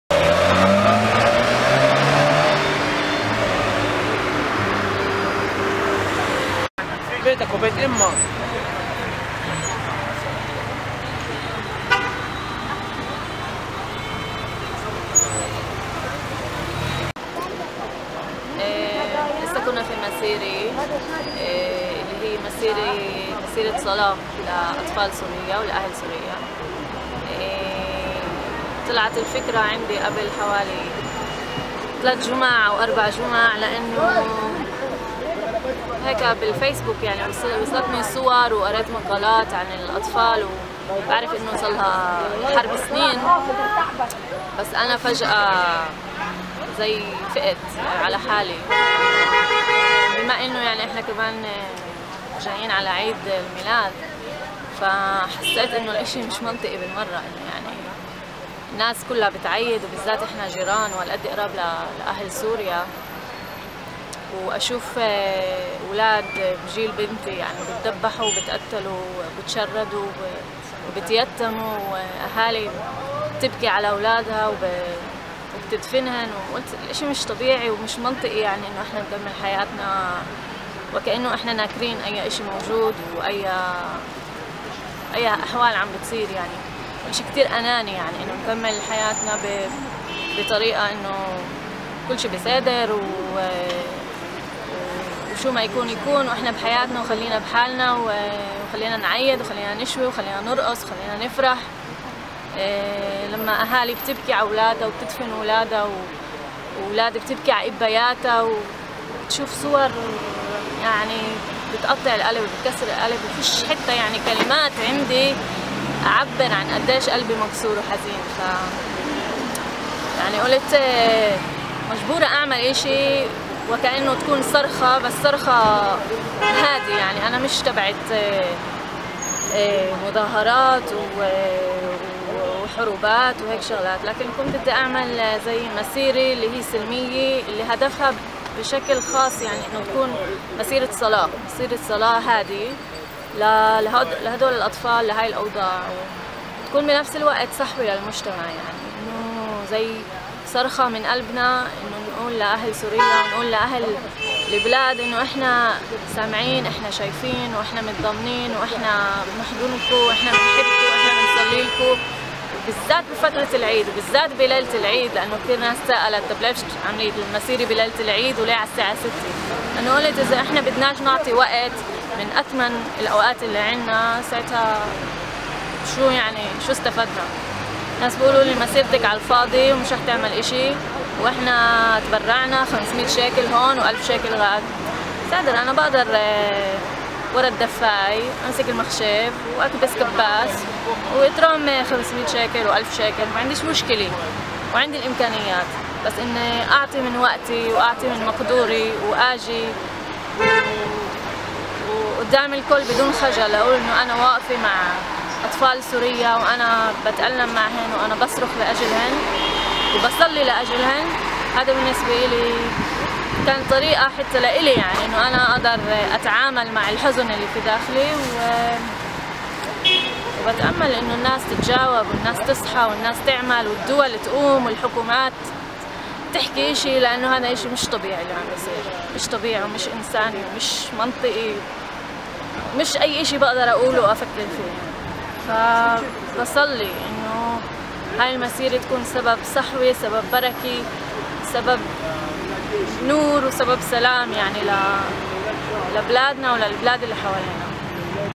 حيفا: مسيرة سلمية في ليلة الميلاد تضامنا مع الشعب السوري
سرحان محاميد
نظمت الطبيبة ناردين سعد صليبا, مساء اليوم السبت , مسيرة سلمية تضامنية مع أبناء الشعب السوري.
وفي حديث مع د. صليبا ذكرت بان اختيار التوقيت , الا وهو ليلة الميلاد المجيد, جاء ليؤكد أهمية بث رسائل الحب والعطاء تعزيزا للإنسانية والتواصل الإنساني والاخوي في ليلة العيد.
وأضافت د. صليبا بانها شعرت بحاجة ملحة منذ 4 أسابيع تقريبا بالتعبير التضامني المغاير مع اهلنا في سوريا، وقد اتتها فكرة تنظيم مسيرة سلمية تنتهي برفع صلوات من اجل الشعب السوري.
وشارك في المسيرة العشرات من أبناء عائلتها , الذين تضرعوا سوية لله من اجل حماية سوريا زمن اجل السلام.